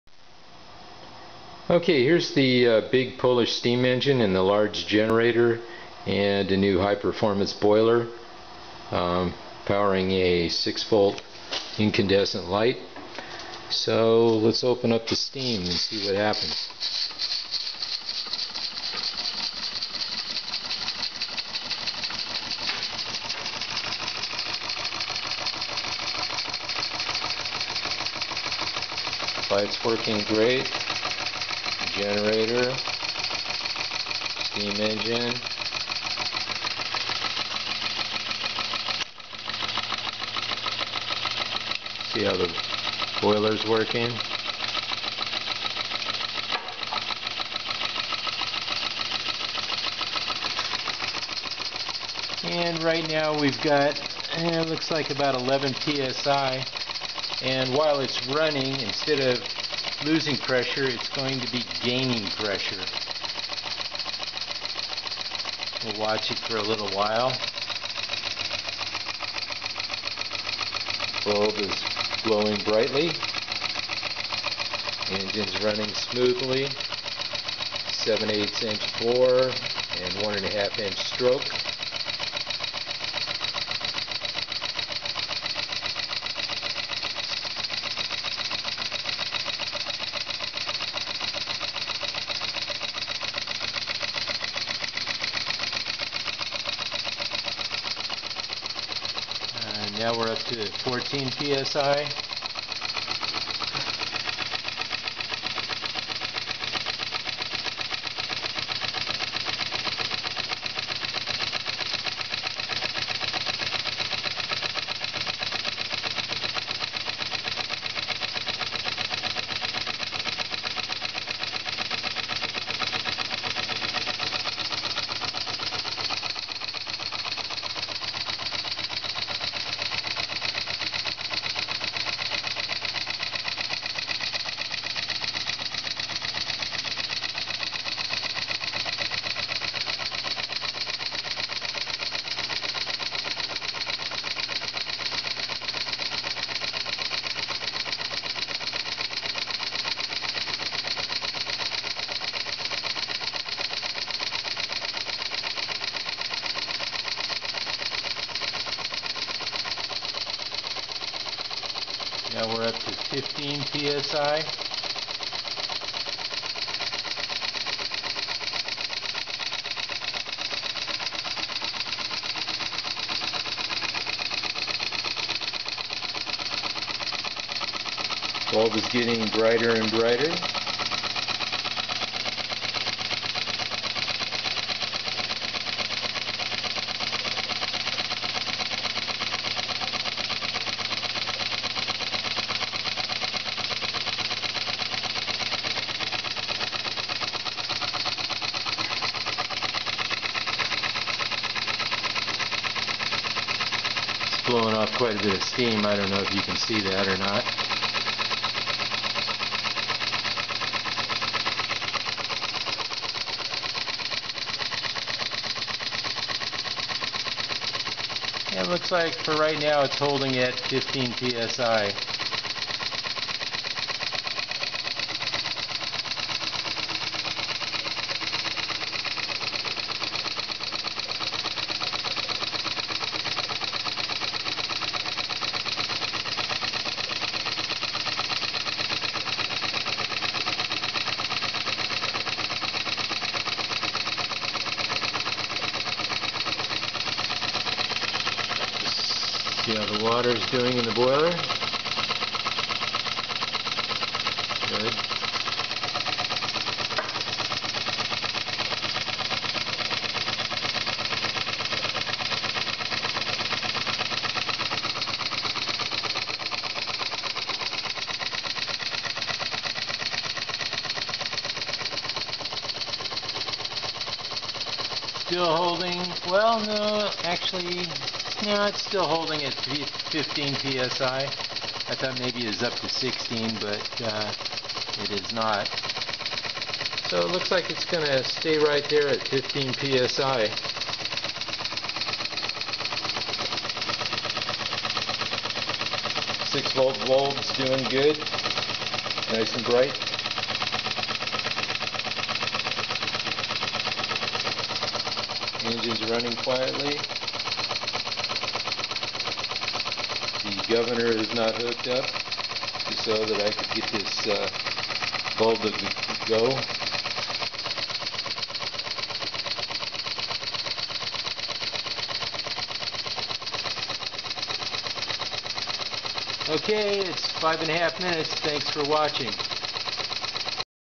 Very Large and Powerful Model Steam Engine
Building up steam pressure while running, without the governor.
Dual heater, 3" Jensen boiler draws over 1,000 watts at full power.  Boiler builds to operating pressure in just 3 minutes.  Run time is 10+ minutes, at full power.  Boiler wattage is fully adjustable from 0-1015.